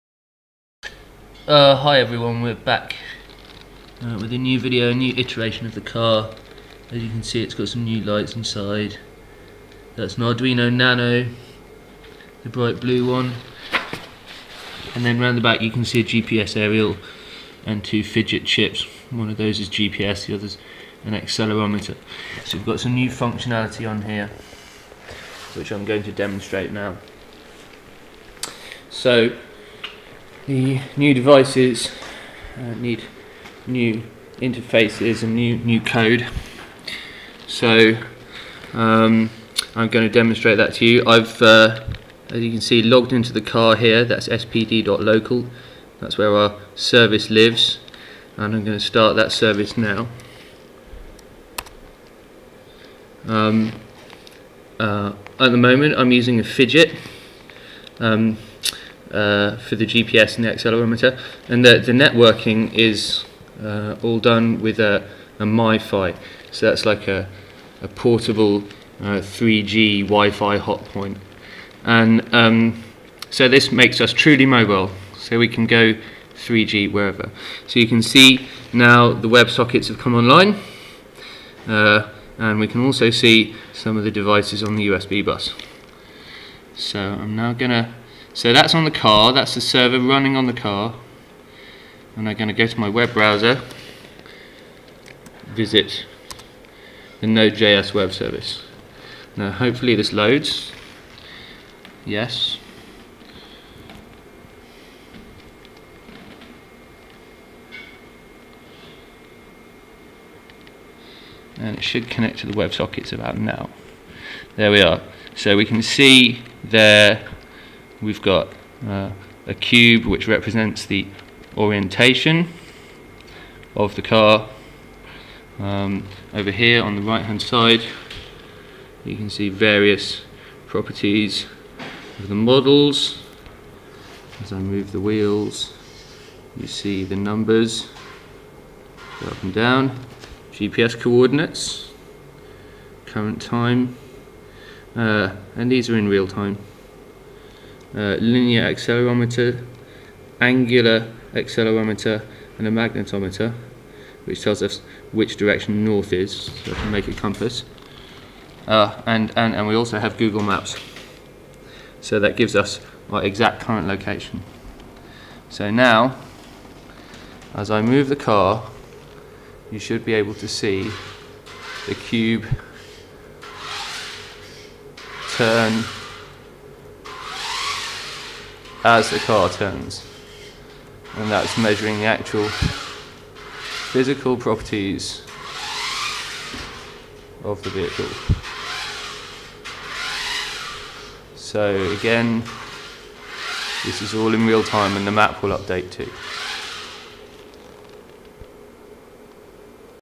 Prototype Raspberry Pi race car
A Raspberry Pi powered, wifi enabled, 1/12 scale Tamiya TL01 prototype autonomous/semi-autonomous race car. The car is controlled via a web page hosted on the car itself, which communicates with a computer using WebSockets over HTTP. The software running on the car is written using Node JS, Backbone and  which allows for autonomous control whilst providing real-time user feedback.

In this video I demonstrate some of the car's latest additions. New features include GPS, linear acceleration, angular acceleration and a 3D compass, all synced direct to browser. More detail is provided in the commentary.

UPDATE! I recently presented the robot at MLOC.JS in Hungary.